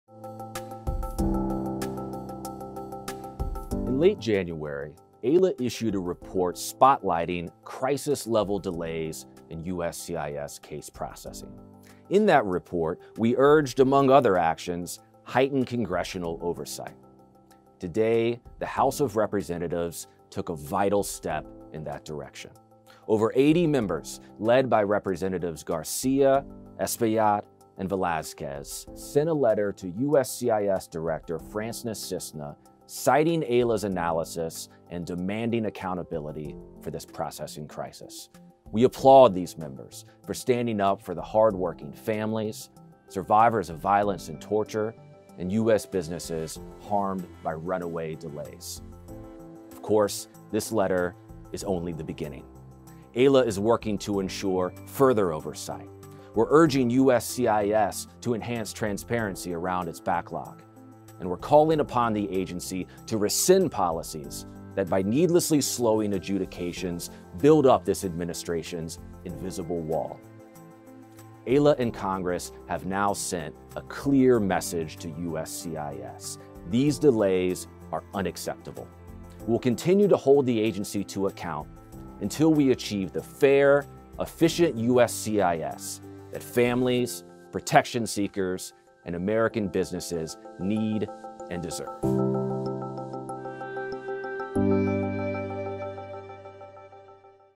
Members of Congress Take Action Based on AILA’s Report
Citing data from AILA National, over 85 Members of Congress call on Director of U.S. Citizenship and Immigration Services, Lee Francis Cissna, to provide reasons for USCIS processing delays and to address the crisis head on.